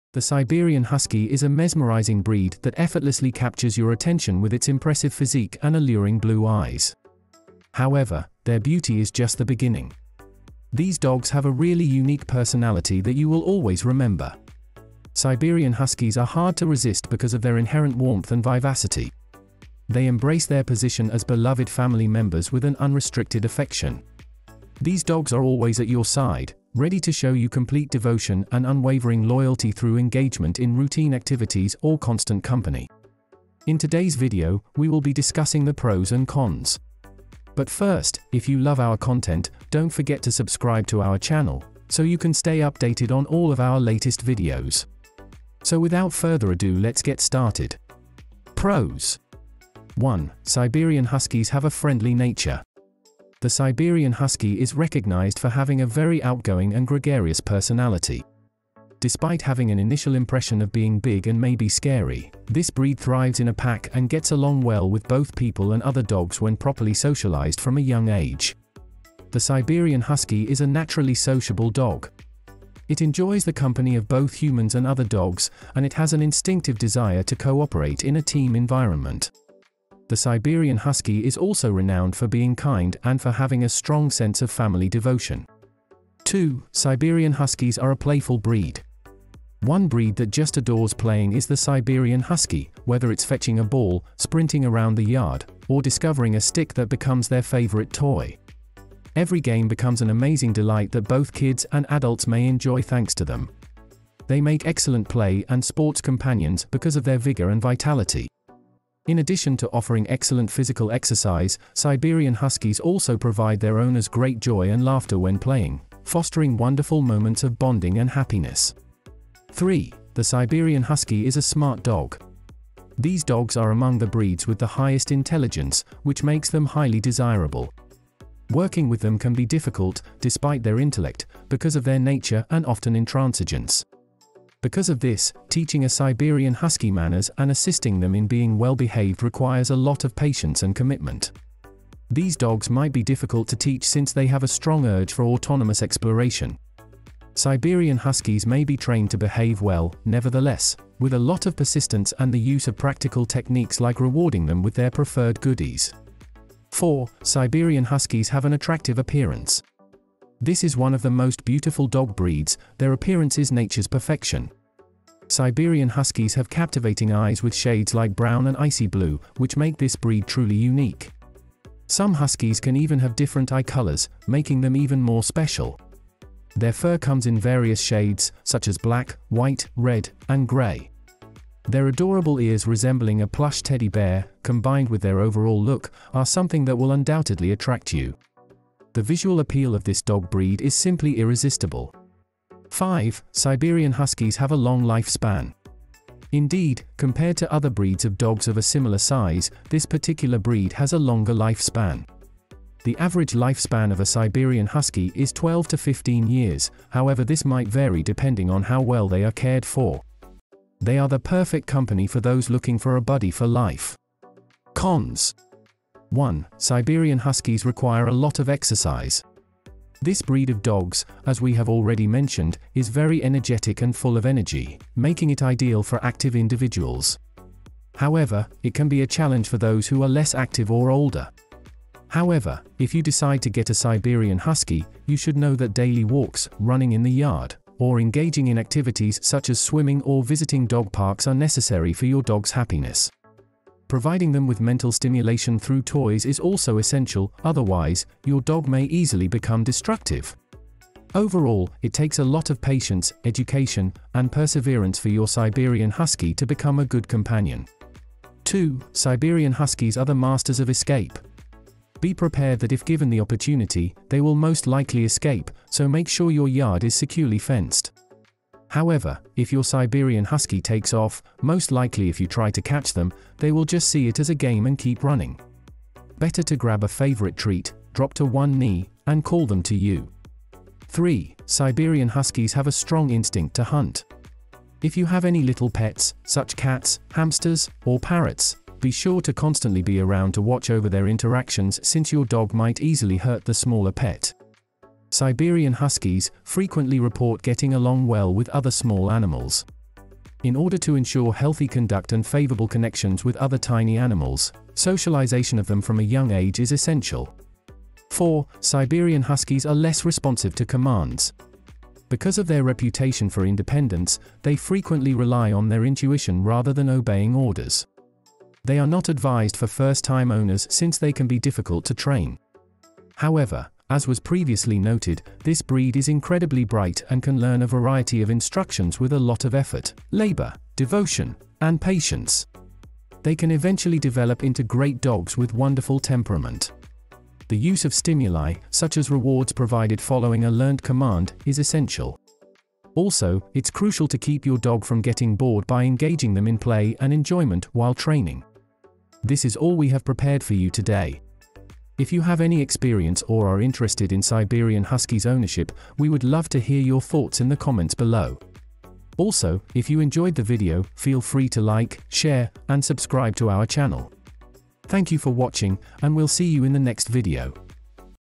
The Pros & Cons Of Owning A Siberian Husky
The Siberian Husky is a mesmerizing breed that effortlessly captures your attention with its impressive physique and alluring blue eyes. However, their beauty is just the beginning. These dogs have a really unique personality that you will always remember. Siberian Huskies are hard to resist because of their inherent warmth and vivacity. They embrace their position as beloved family members with an unrestricted affection. These dogs are always at your side, ready to show you complete devotion and unwavering loyalty through engagement in routine activities or constant company. In today's video, we will be discussing the pros and cons.

0:00 intro
0:50 - 1. Friendly and loyal | Pros
1:26 - 2. Siberian Huskies are a playful breed| Pros
1:59 - 3. The Siberian Husky is a smart dog | Pros
2:37 - 4. Siberian Huskies have an attractive appearance | Pros
3:12 - 5. Siberian Huskies have a long lifespan | Pros

3:35 - 1. Siberian Huskies require a lot of exercise | Cons
4:18 - 2. Siberian Huskies are the masters of escape | Cons
4:40 - 3. Siberian Huskies have a strong instinct to hunt | Cons
5:08 - 4. Siberian Huskies are less responsive to commands | Cons